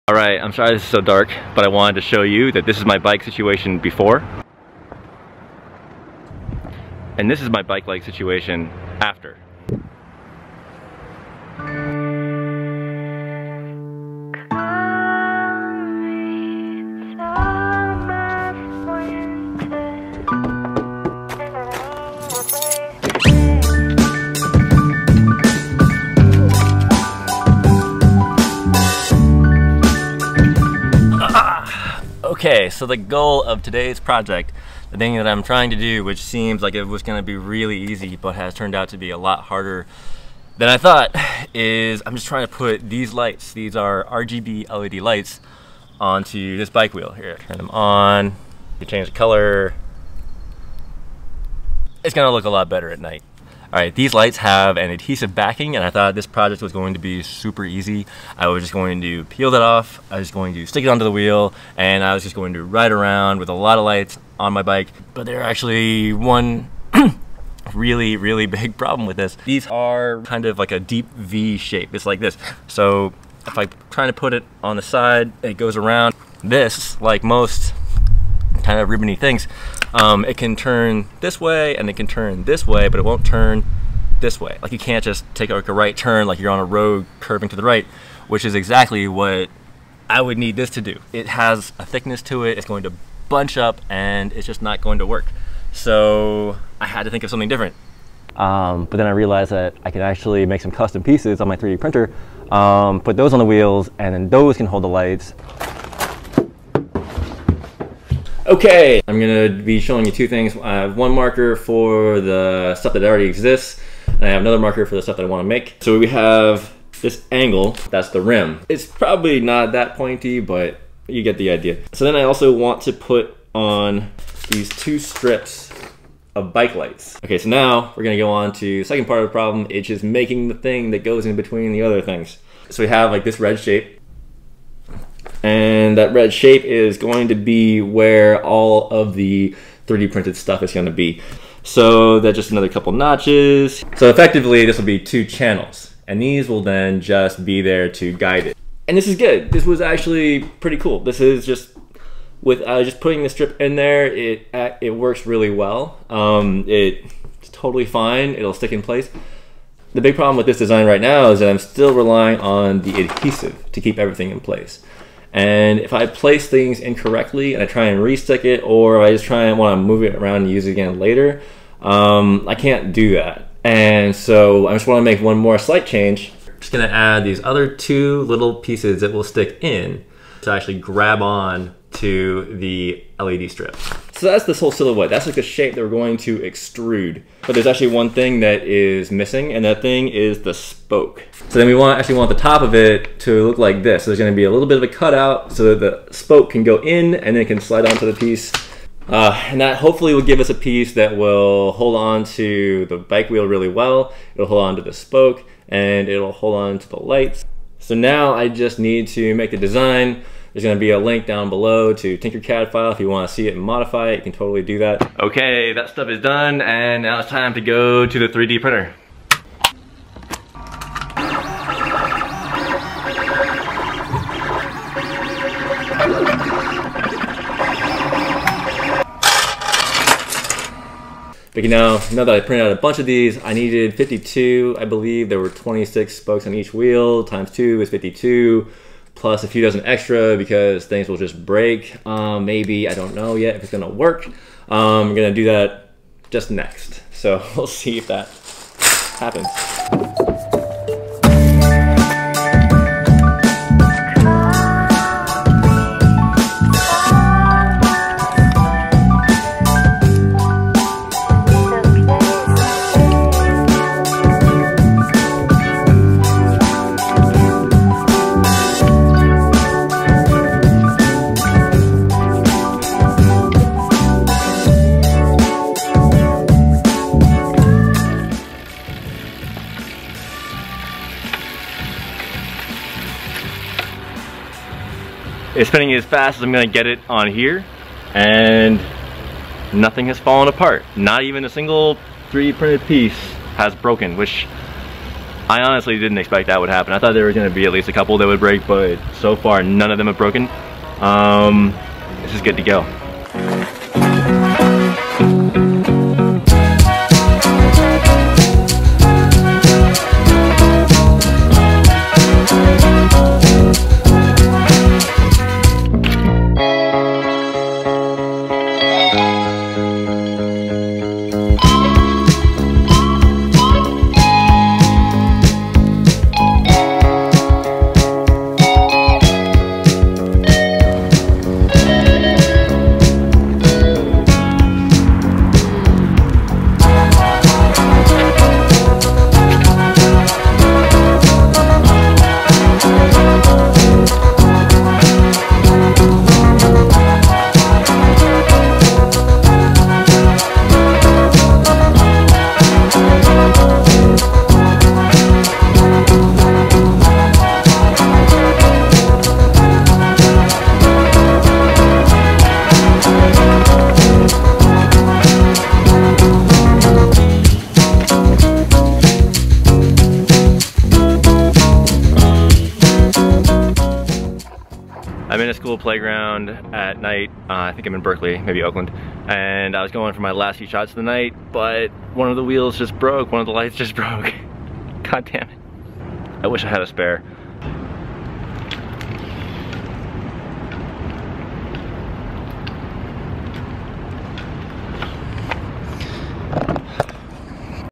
Putting 200 RGB LED lights onto bike wheels
I put 8 meters of RGB LED strips onto my bike wheels for a total of 200 LEDs. Super bright, even a bit blinding, but very fun.

PLA filament